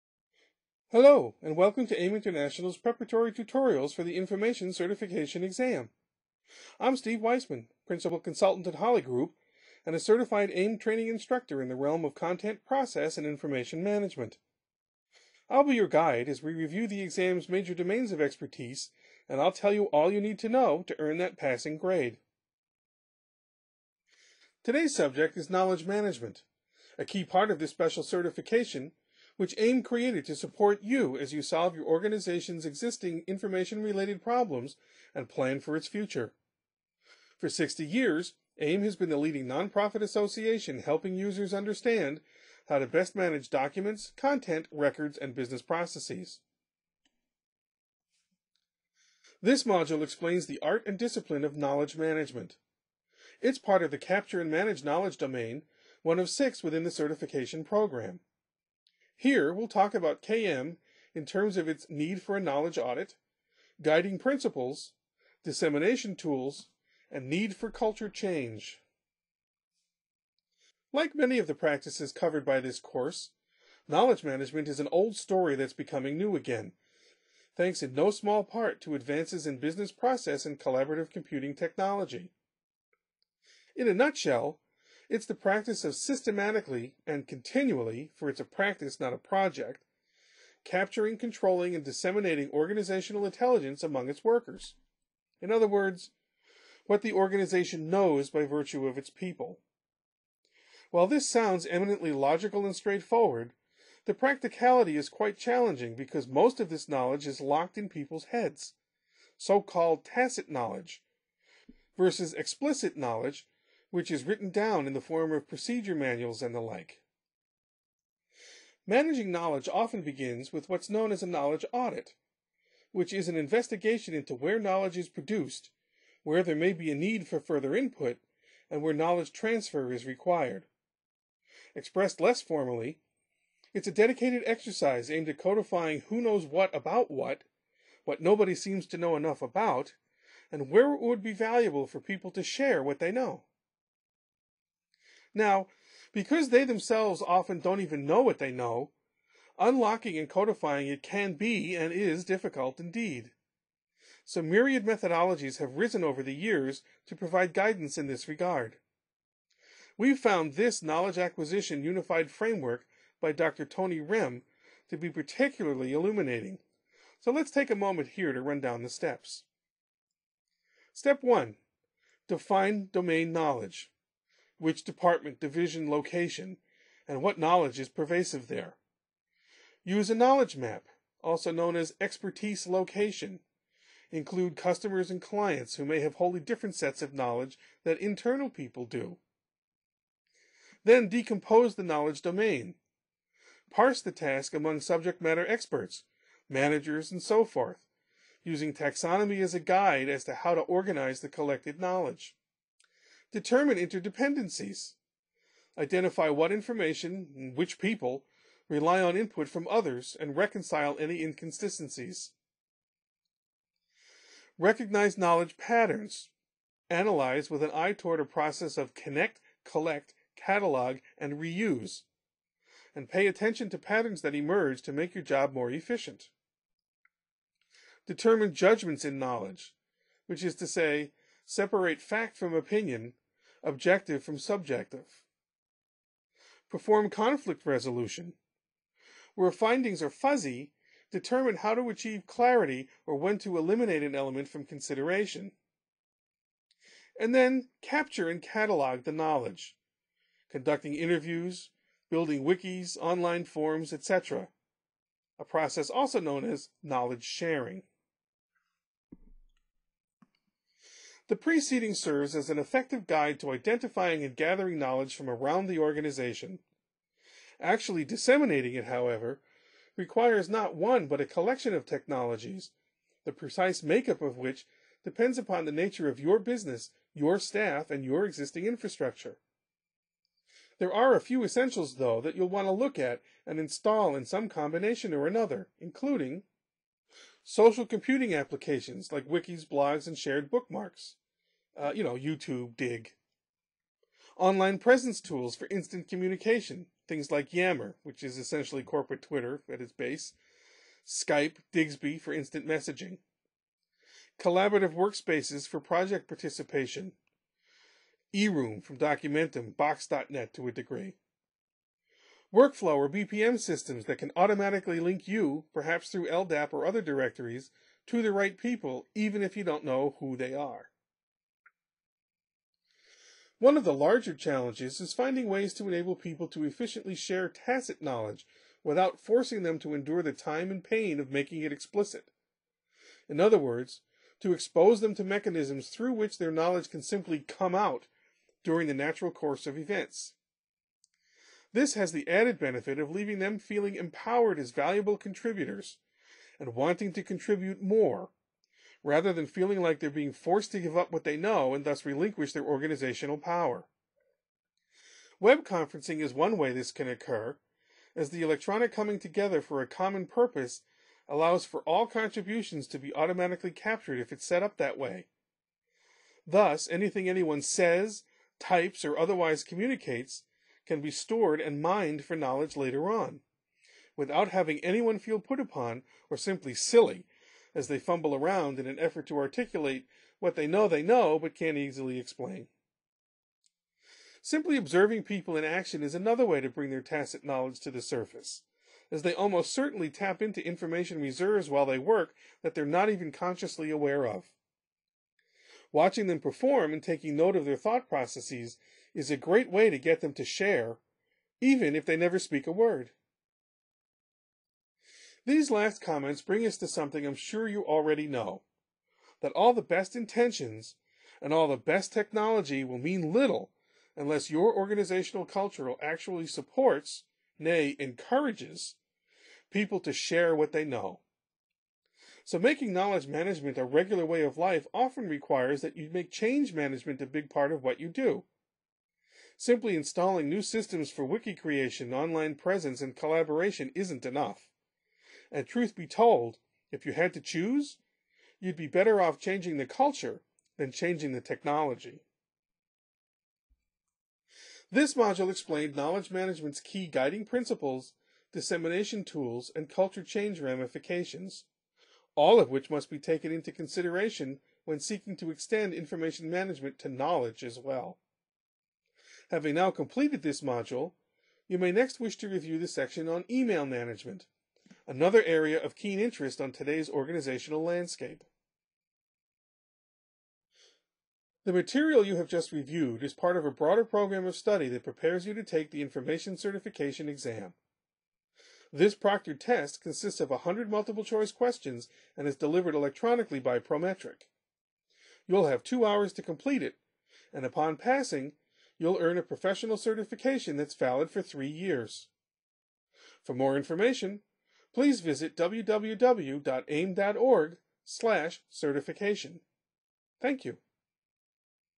2.3.1 KM Principles Practices and Primary Tools
This video describes the key concepts associated with knowledge management including the knowledge audit, guiding principles, and knowledge dissemination tools.